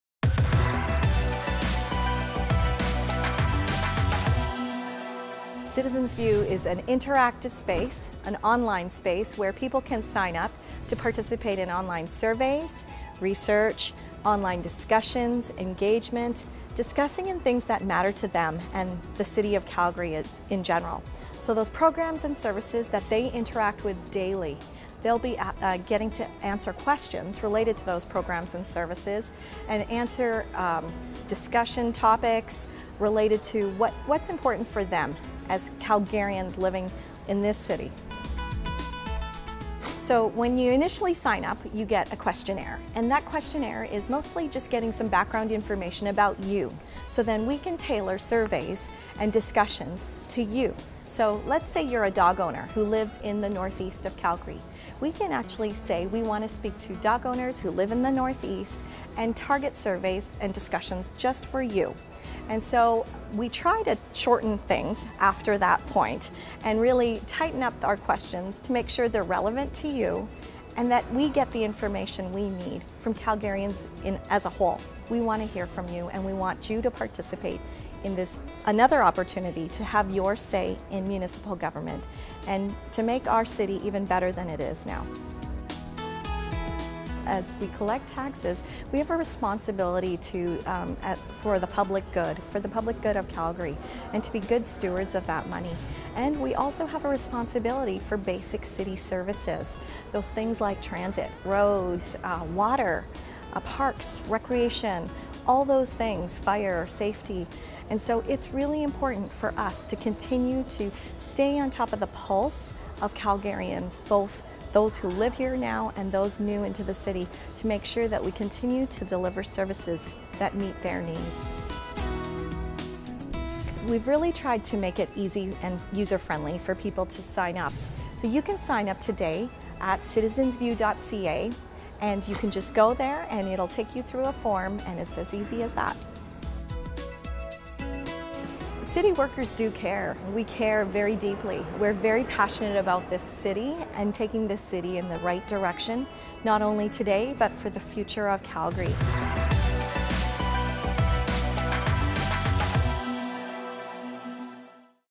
City Matters: Citizens' View
Allocating tax revenues towards various programs is a big responsibility. Here is your opportunity to state your opinions through the City's Citizens' View Program.